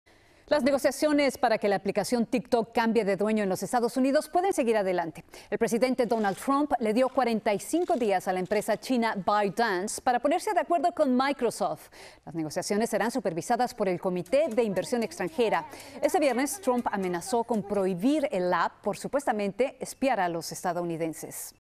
Tik Tok podría cambiar de dueño en Estados Unidos | Noticias Telemundo
Video oficial de Noticias Telemundo. El presidente Donald Trump le dio 45 días a la empresa China 'Bytedance' para ponerse de acuerdo con Microsoft, las negociaciones serán supervisadas por el comité de inversión extranjera.